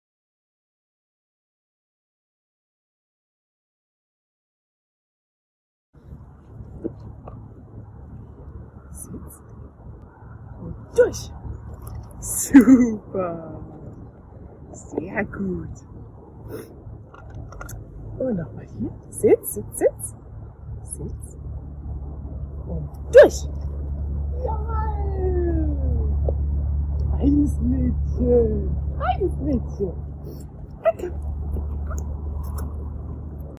Laya | Ab durch den Tunnel!
Heute gibt es mal wieder einen Behind-the-Scenes Einblick in Layas Leben. Beim Spazierengehen auf Sylt haben wir diese Tonne entdeckt - eine wunderbare Möglichkeit, ein neues Kunststück zu lernen! "Durch" ist ihr neues Lieblingskommando 🐶🧡